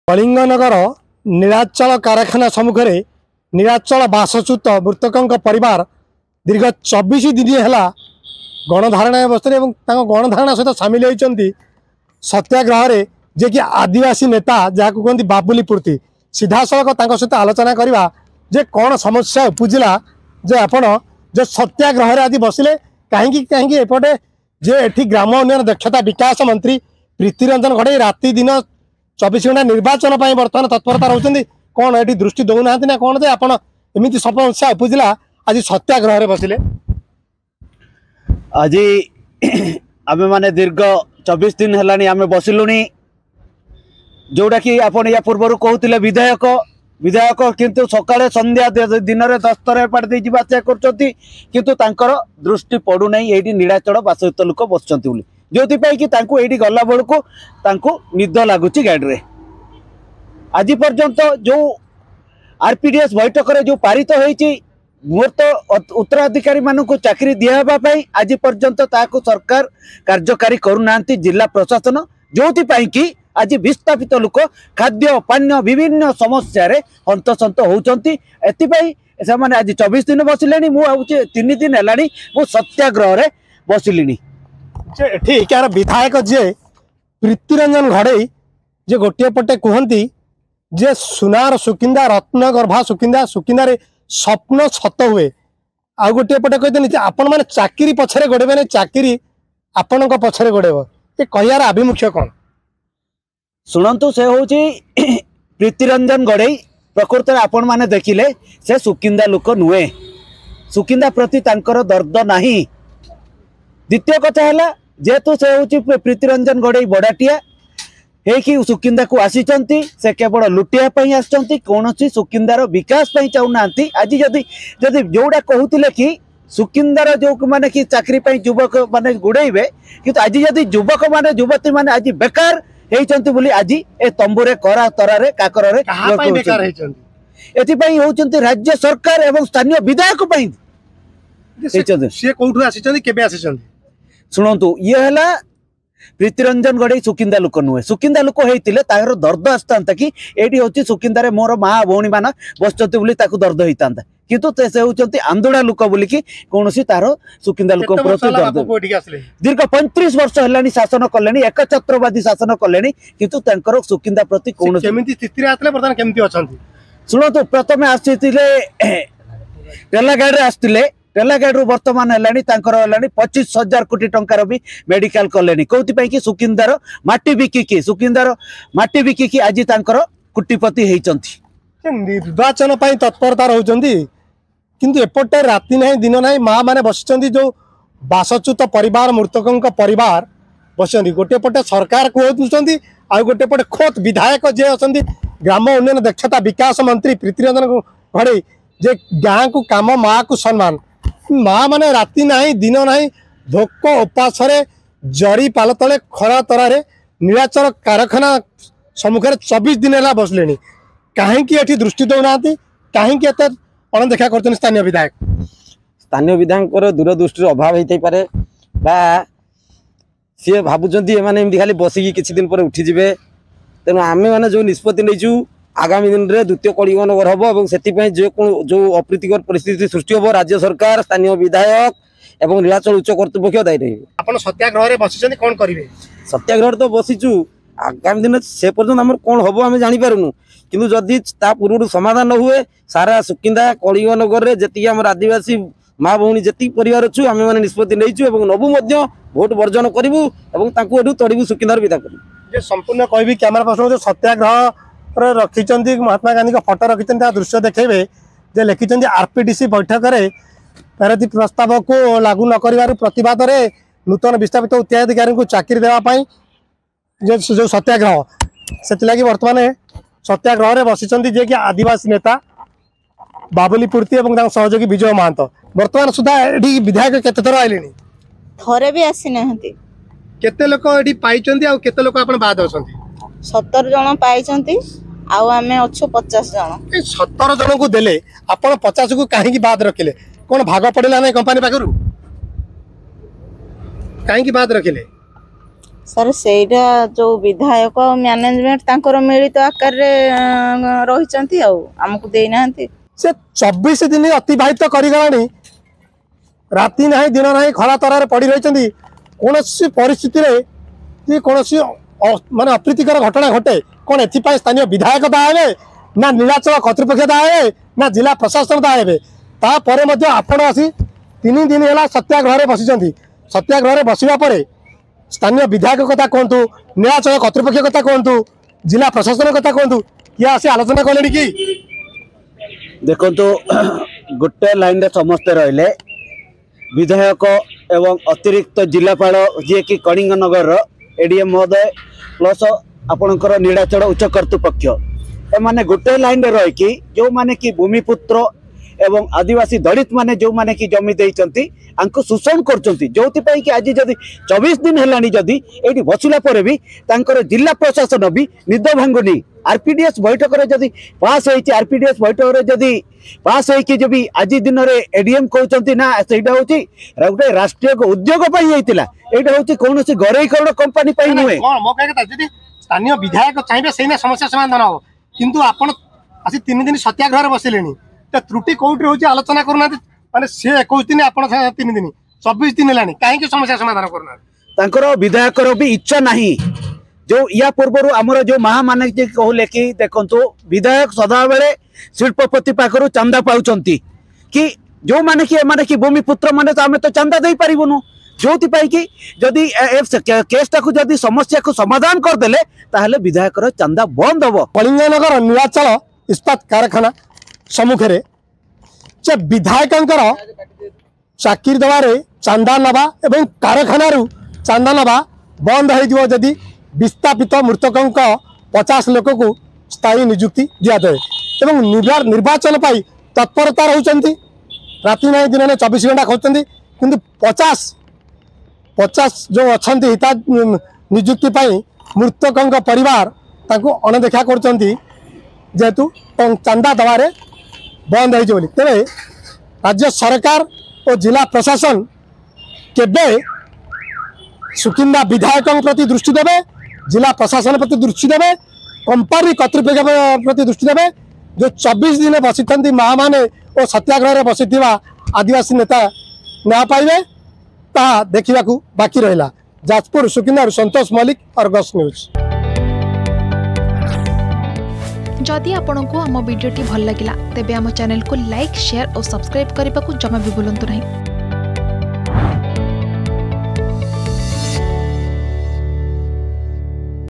ନିଯୁକ୍ତି ଦାବିରେ ନୀଳାଚଳ କାରଖାନା ସମ୍ମୁଖରେ ଧାରଣା || Protest || Jajpur
ଯାଜପୁର କଳିଙ୍ଗନଗର ନୀଳାଚଳ କାରଖାନା ସମ୍ମୁଖରେ ବିସ୍ଥାପିତ ବାସଚ୍ୟୁତ ପରିବାର ସ୍ଥାୟୀ ନିଯୁକ୍ତି ଦାବିରେ ଦୀର୍ଘ 24 ଦିନ ହେବ ଧାରଣାରେ ବସିଛନ୍ତି ।